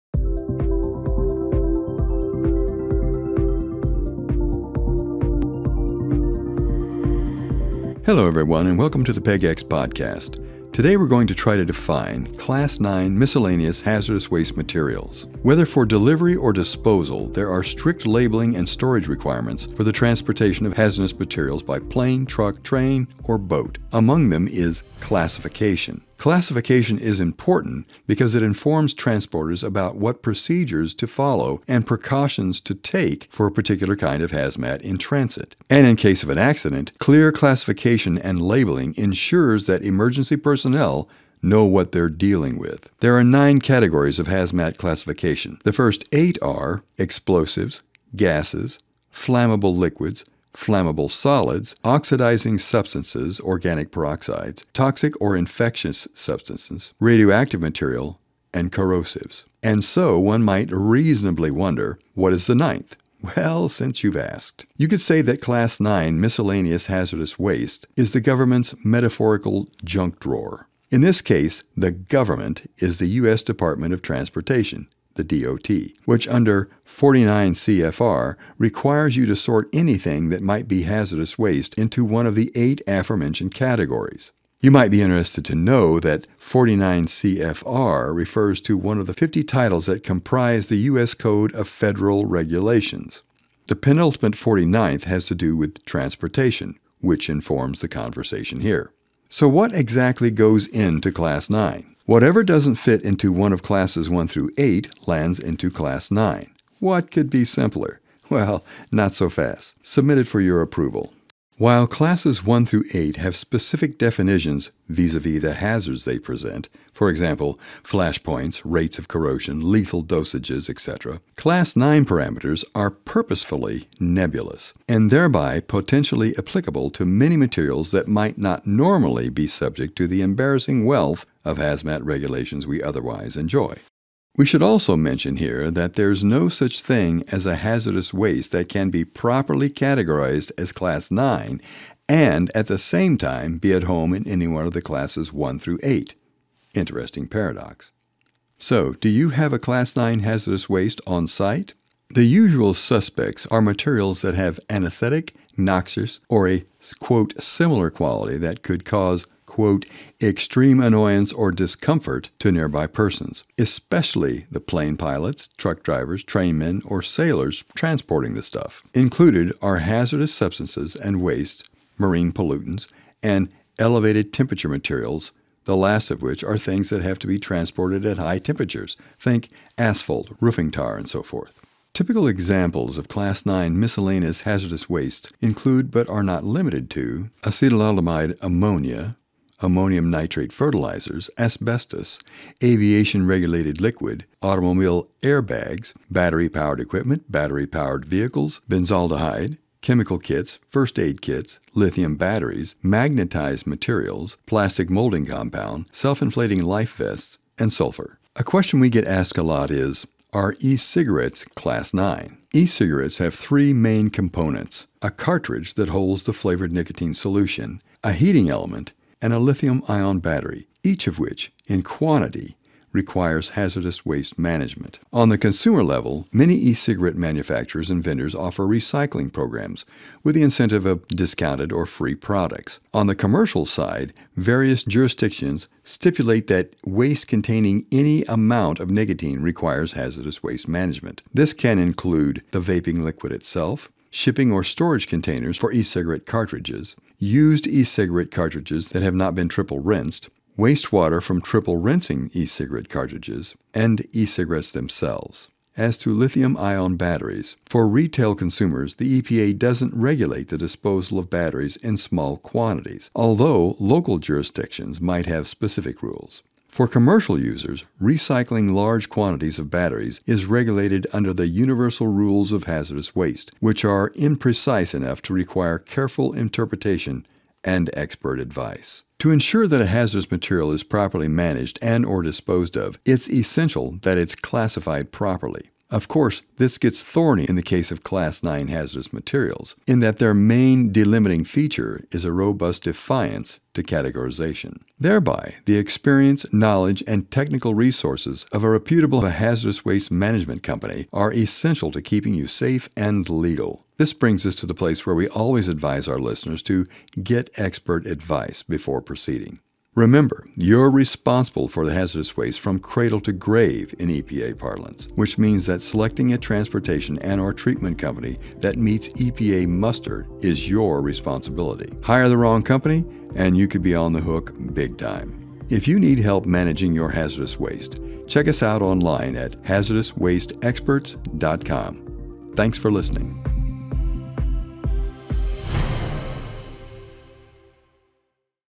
Class 9 Miscellaneous Hazardous Materials
Whether for delivery or disposal, there are strict labeling and storage requirements for the transportation of hazardous materials by plane, truck, train, or boat. Among them is classification.

Classification is important because it informs transporters about what procedures to follow and precautions to take for a particular kind of hazmat in transit. And in case of an accident, clear classification and labeling ensure that emergency personnel know what they’re dealing with.

In this video, we attempt to define class 9 miscellaneous hazardous waste materials.